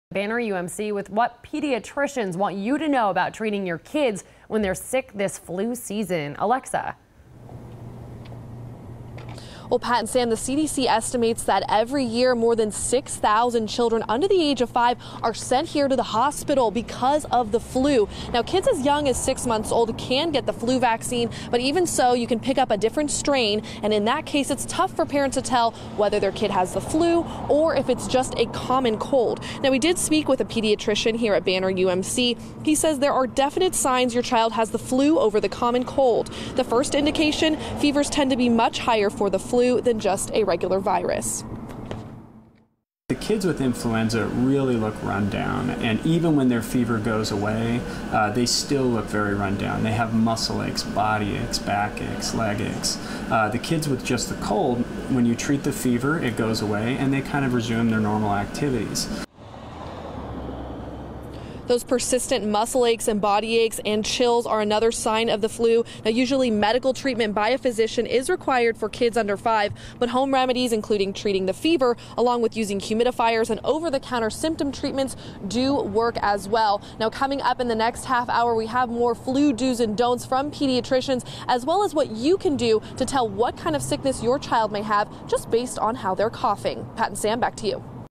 Protecting your kids during flu season
Here's what you know to defend your kids against illness this flu season.